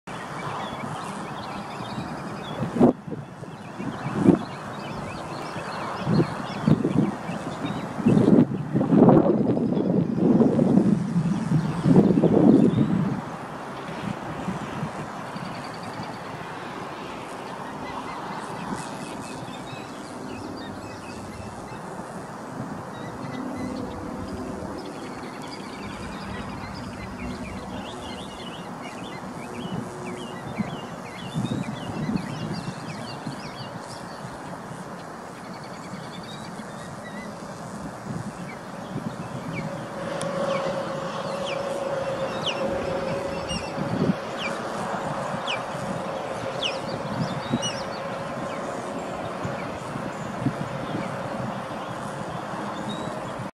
Bald Eagle off Edgemoor road in Oak Ridge, TN
The noises are from the osprey who are right over me in nest on high power line tower.